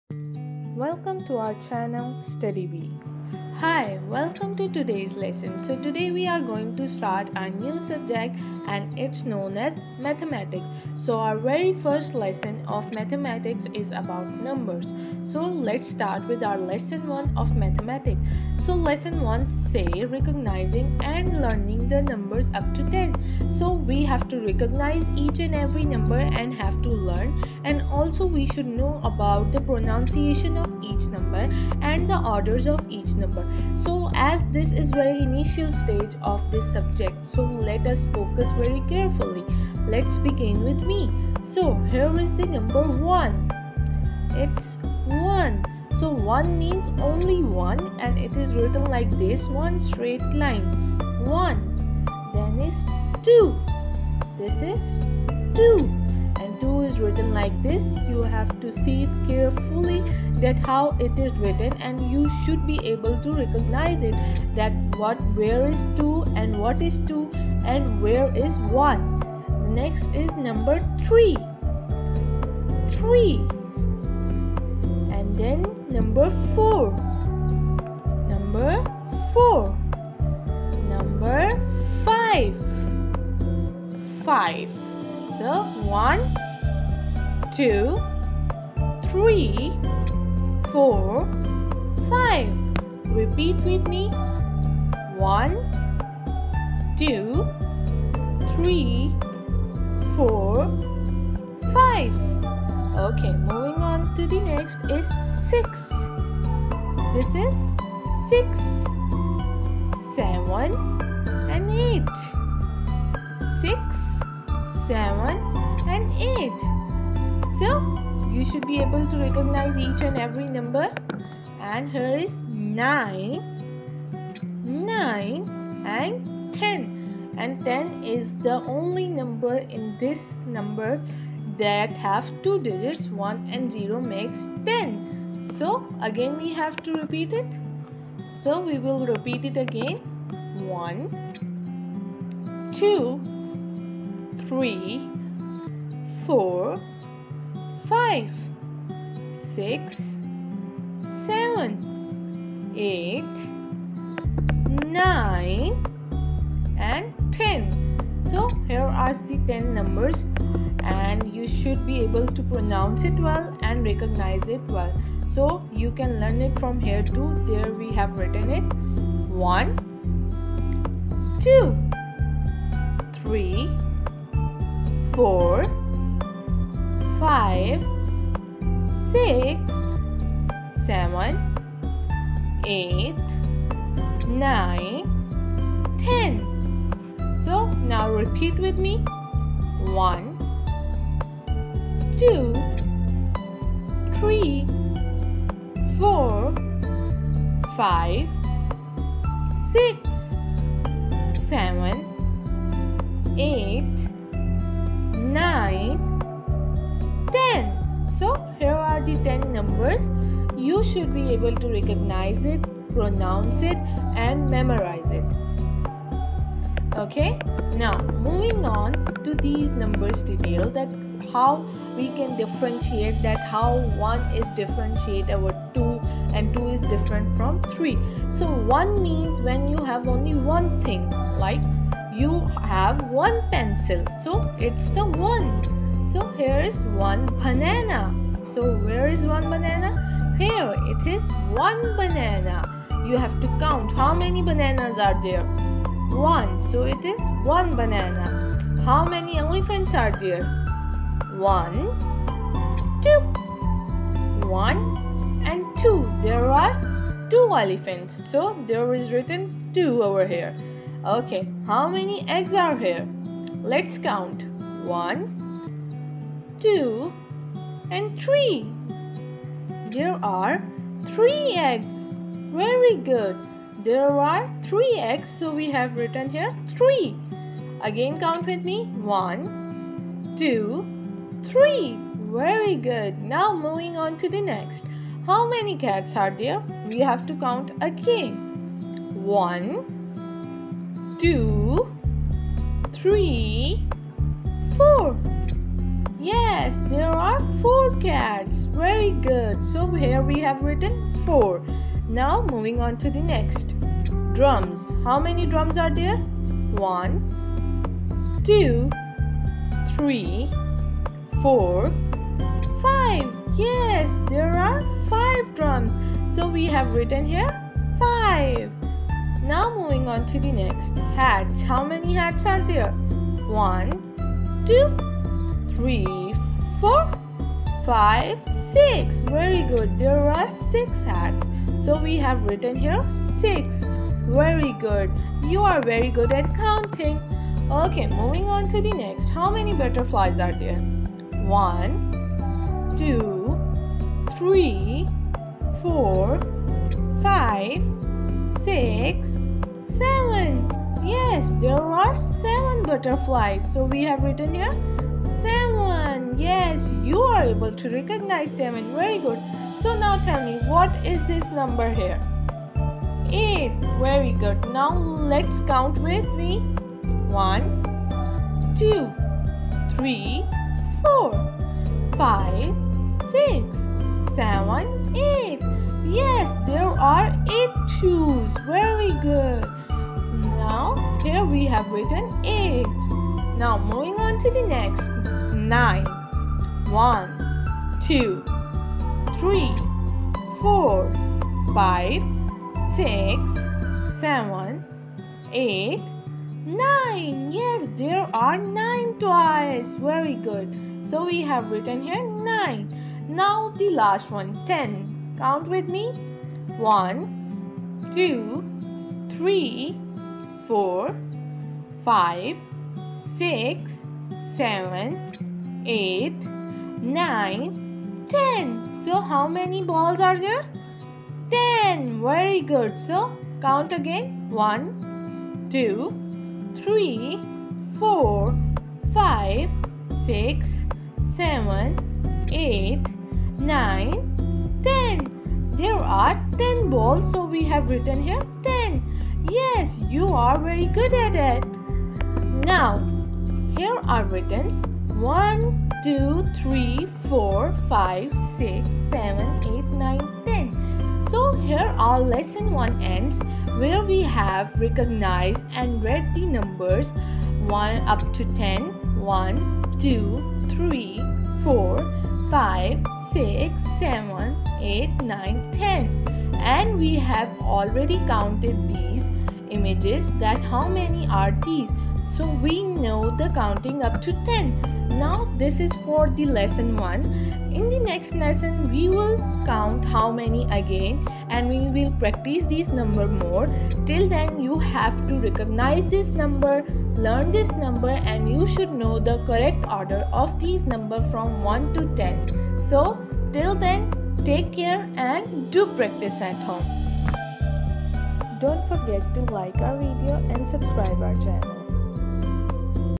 Cambridge Grade 1 Maths Lesson 01 | Study Bee
Welcome to our hive 🐝
Today we are so thrilled to come to you with our first video for a new subject. Yes, we are finally starting mathematics and we can't wait for you all to join us through all the new lessons. In our first lesson today we will identify numbers.

You can learn more from us through Instagram with our daily vocabulary posts 🐝